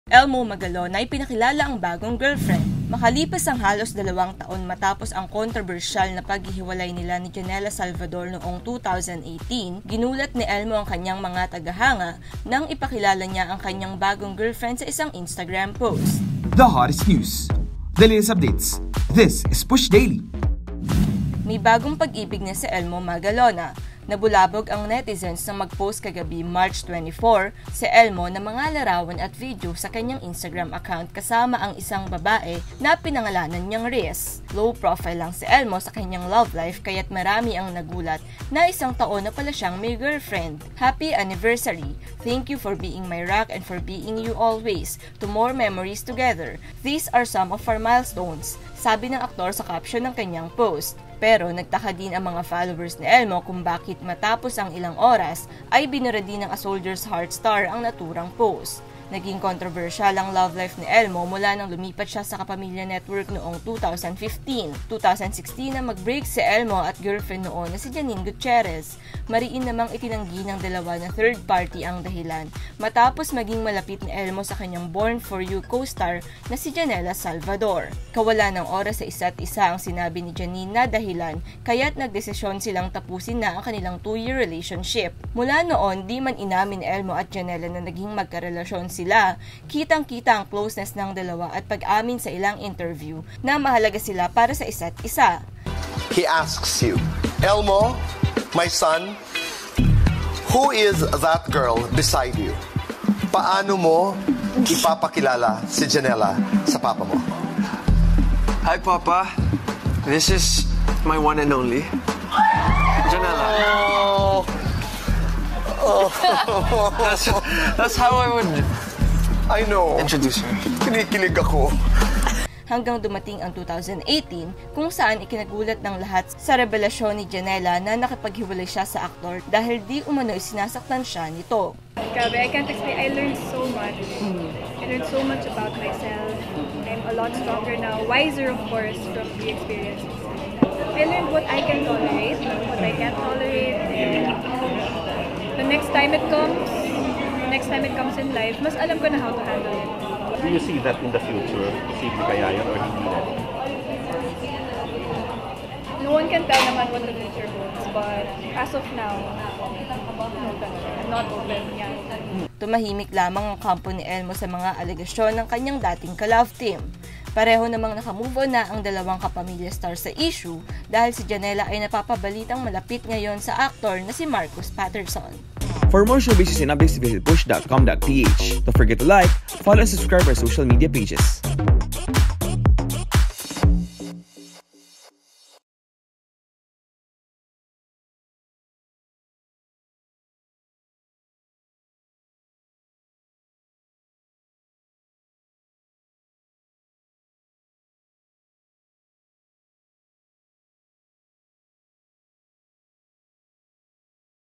Elmo Magalona, ipinakilala ang bagong girlfriend | PUSH Daily
Makalipas ang halos dalawang taon mula nang mahiwalay ito kay Janella Salvador ay ipinakilala na ni Elmo Magalona ang kanyang bagong girlfriend. (Read

For more updates, just go to push.com.ph!